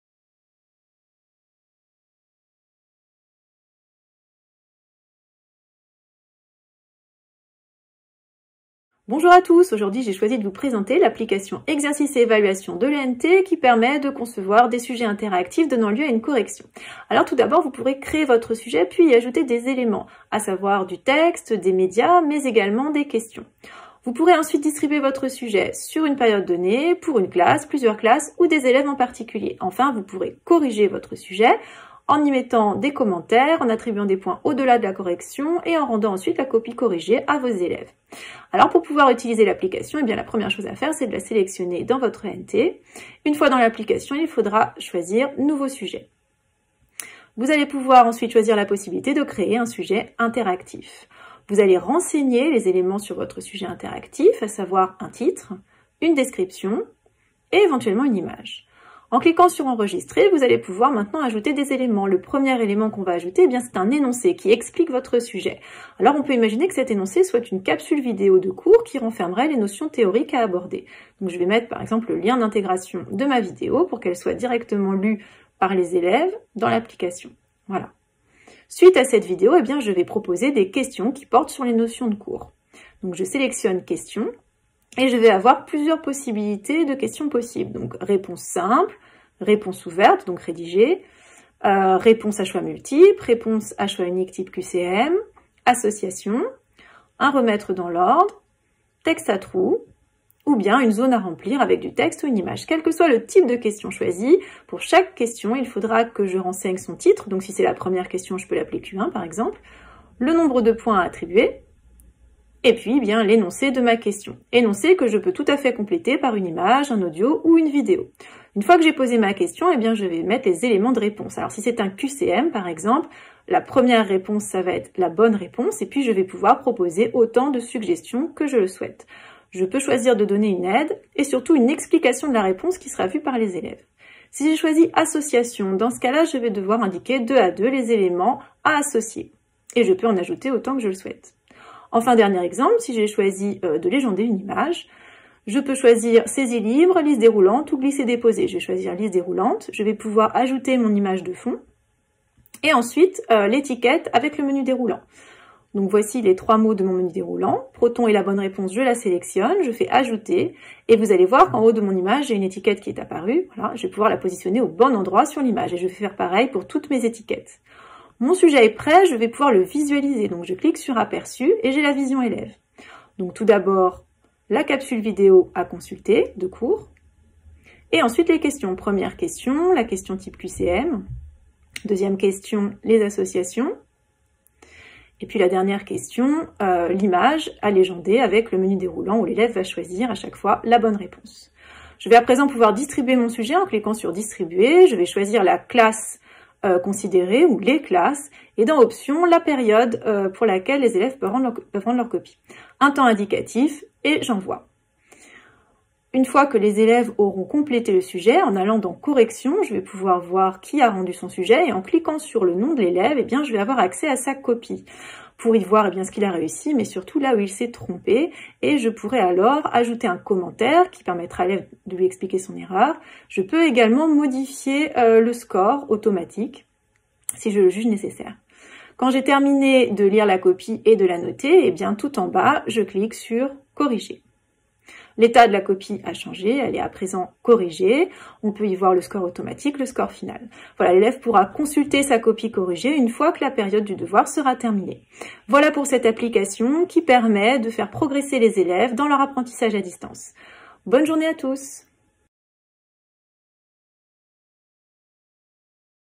Utiliser l'application "exercices et évaluations" dans l'ENT
Aujourd'hui Audrey Campbell, professeure, nous conseille sur l'utilisation de  l'application "exercices et évaluations" dans l'ENT - Dane Versailles - avril 2020